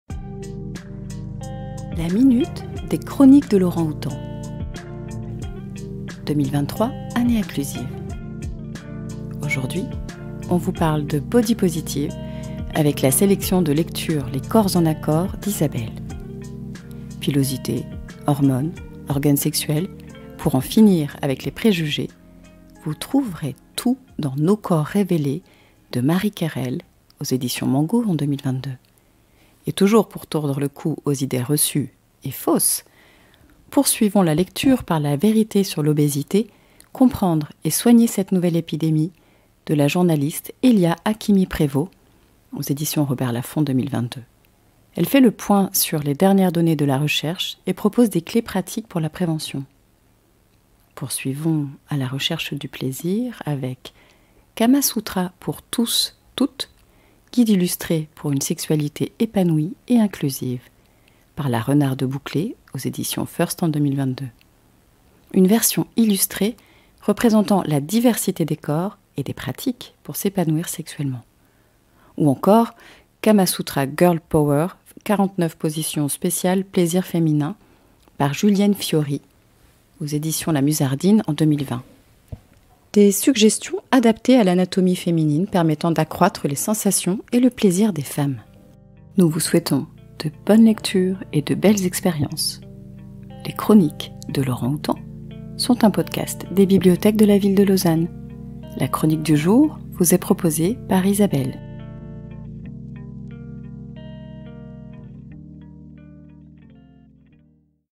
Lire en body positive - Sélection documentaires | Podcast #72
Dans le cadre de la thématique "2023 année inclusive" dans nos bibliothèques, découvrez une sélection de documentaires body positive élaborée par Isabelle.

Nos corps révélés : cerveau, hormones, pilosité, organes sexuels, poitrine : pour en finir avec les préjugés, par Marie Cayrel
La vérité sur l'obésité : comprendre et soigner cette nouvelle épidémie, par Hélia Hakimi-Prévot
Kama sutra pour tous.tes : guide illustré pour une sexualité épanouie et inclusive, par La renarde bouclée
Kama sutra girl power : 49 positions spécial plaisir féminin, par Julienne Fiori